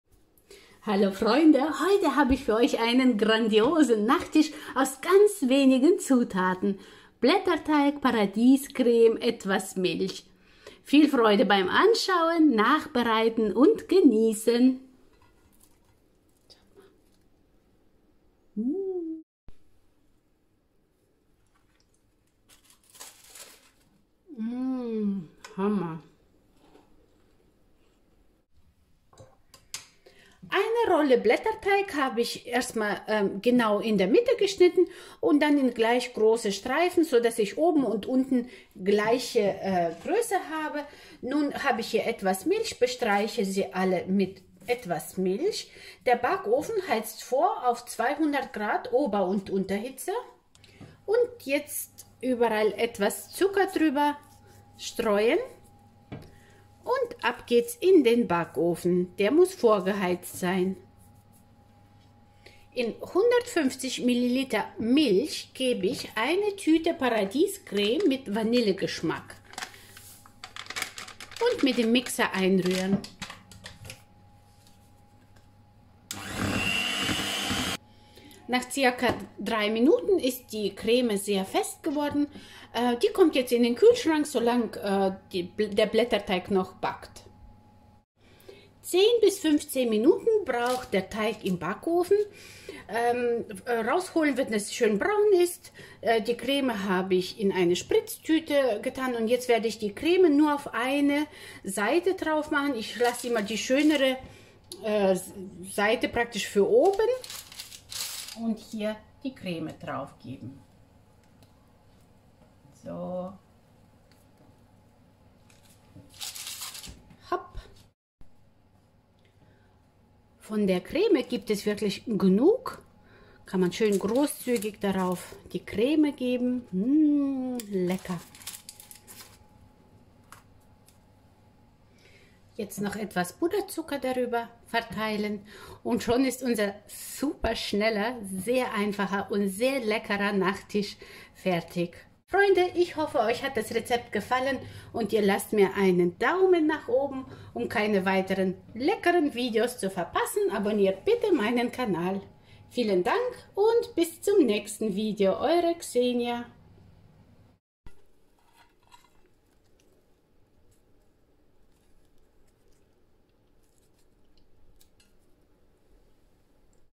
Blätterteig-Taschen mit Paradiescreme 💕 himmlisch lecker, schnell und aus wenigen Zutaten
Blätterteigtaschen mit Paradiescreme

275 gr Blätterteigrolle
2 EL + 150 ml Milch
2 EL Zucker
1 Tüte Paradiescreme
1 EL Puderzucker

Gebacken wird bei 200 Grad, Ober-und Unterhitze, 10-15 Minuten

Facebook-Gruppe: Zu Tisch bei Ksenia